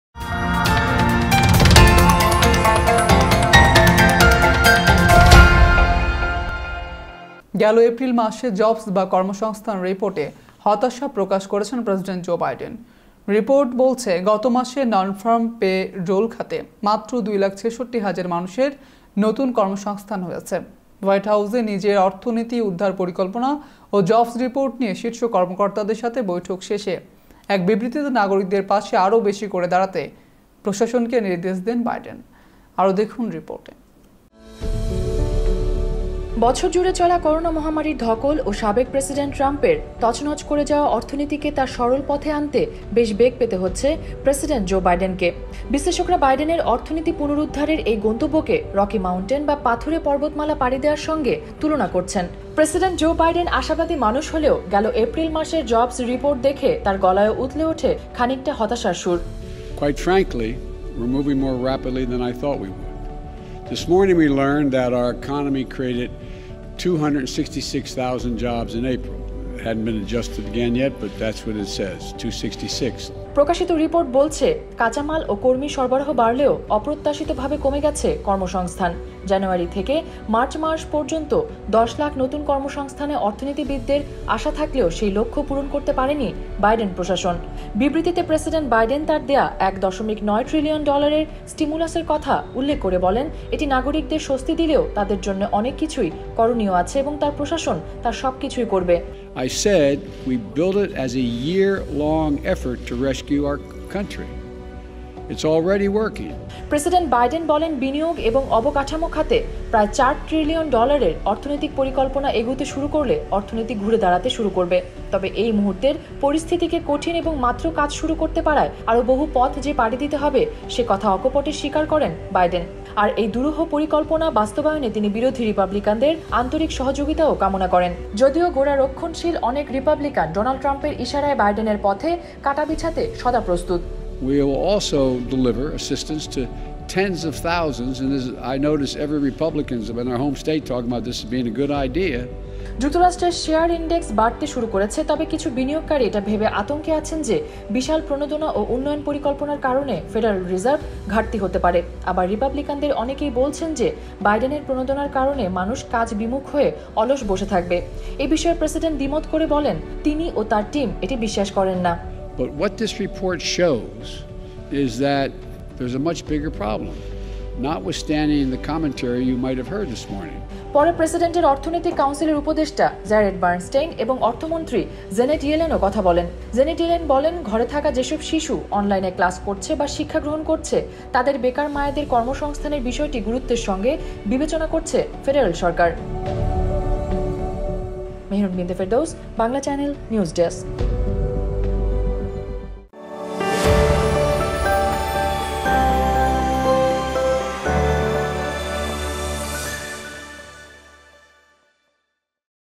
এপ্রিলের জবস রিপোর্টে হতাশ প্রেসিডেন্ট জো বাইডেন, বললেন- অর্থনৈতিক পথ পুনরুদ্ধারে অনেক পথ বাকি।
🌟 বিস্তারিত: গেল এপ্রিল মাসের জবস বা কর্মসংস্থান রিপোর্টে হতাশা প্রকাশ করেছেন প্রেসিডেন্ট জো বাইডেন। রিপোর্ট বলছে গতমাসে ননফার্ম পে রোল খাতে মাত্র ২ লাখ ৬৬ হাজার মানুষের নতুন কর্মসংস্থান হয়েছে। হোয়াইট হাউসে নিজের অর্থনীতি উদ্ধার পরিকল্পনা ও জবস রিপোর্ট নিয়ে শীর্ষ কর্তাদের সাথে বৈঠক শেষে এক বিবৃতিতে নাগরিকদের পাশে আরও বেশি করে দাঁড়াতে প্রশাসনকে নির্দেশ দেন বাইডেন।

🌟  যোগাযোগের ঠিকানা:
Bangla Channel